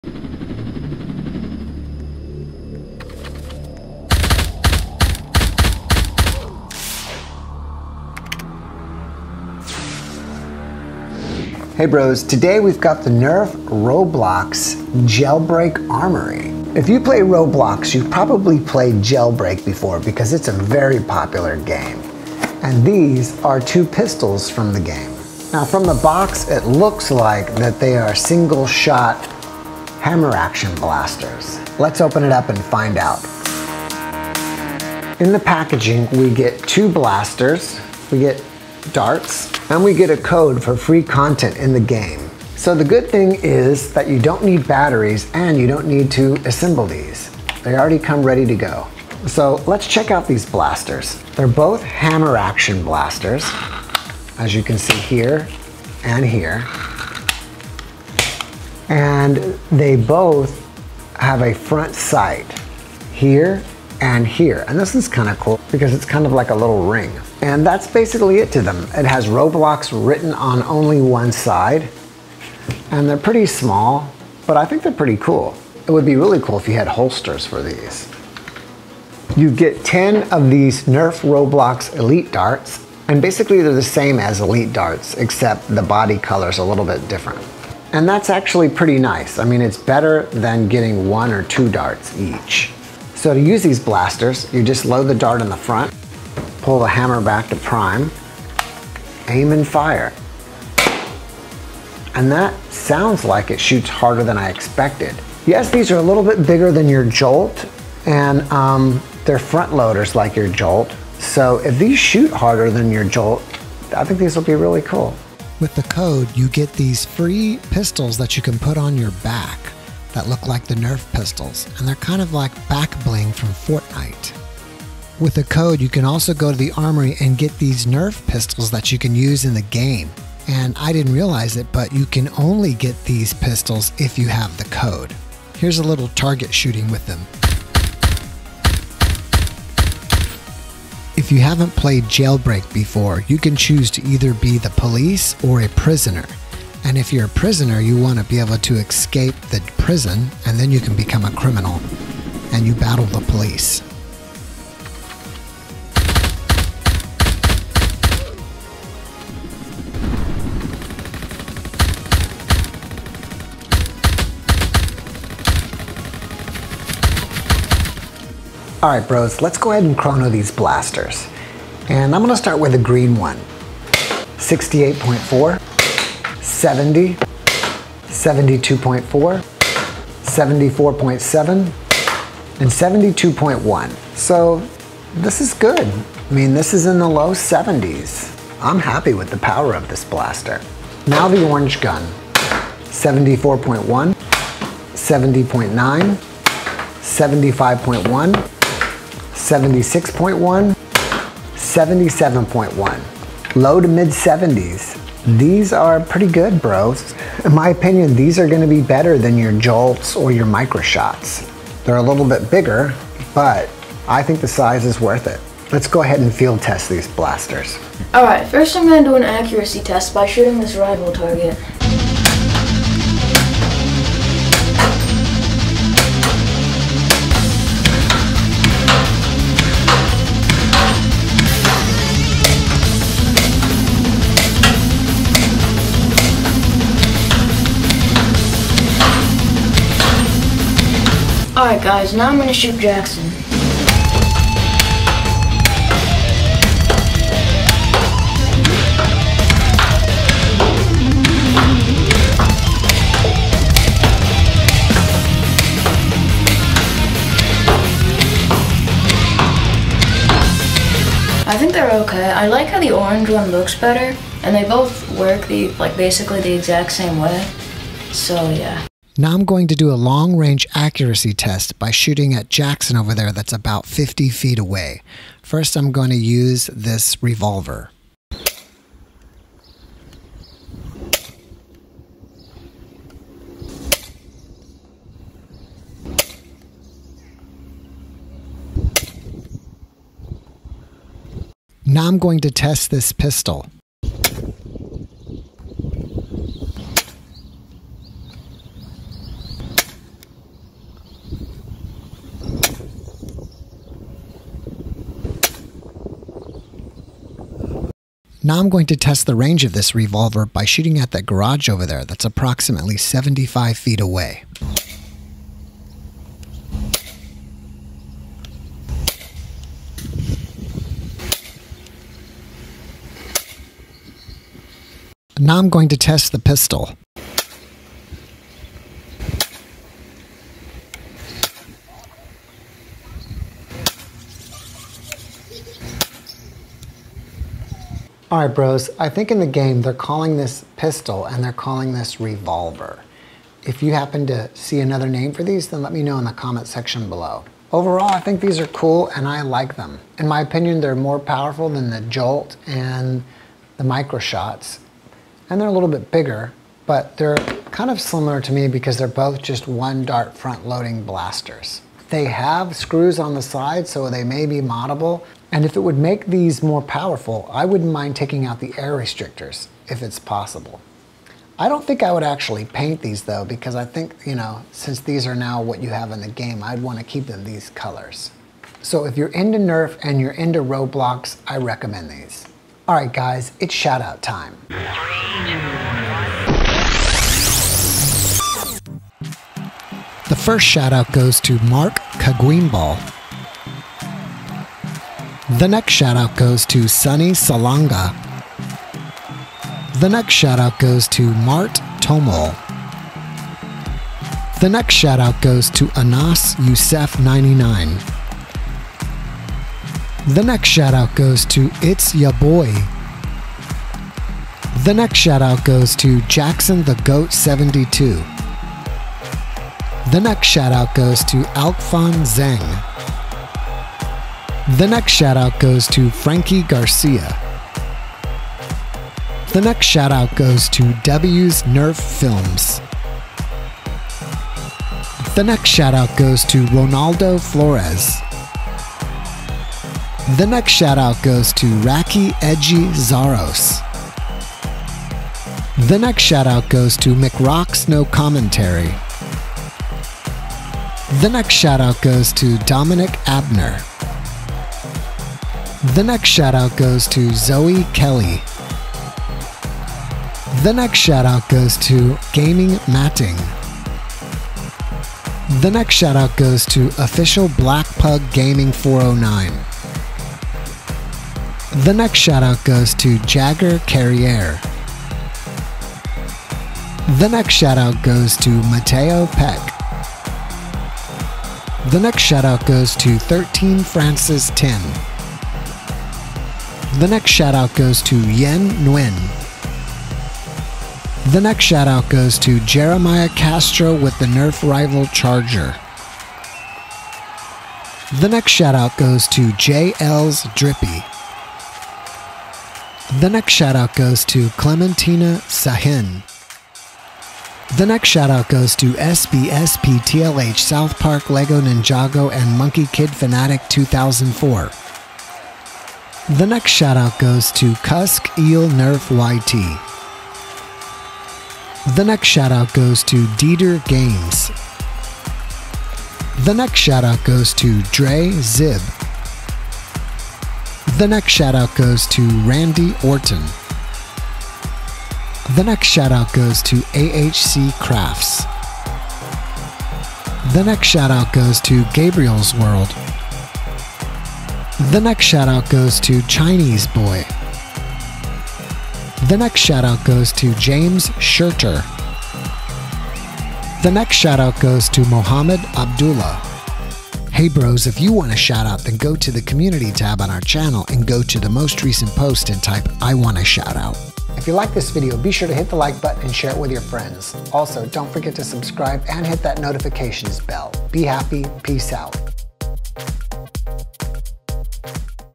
NEW Nerf Roblox Jailbreak Blasters!
This nerf gun set comes with  2 nerf dart blasters in this ROBLOX JAILBREAK: ARMORY set and they take their inspiration from the ROBLOX game Jailbreak. This nerf gun pack also includes a special code that allows you to redeem an exclusive virtual item on ROBLOX! These nerf blasters are hammer-action priming and the set comes with 10 Official NERF ELITE foam darts. With 2 nerf guns, you can have a nerf war with a friend or you can use both in a roblox nerf battle.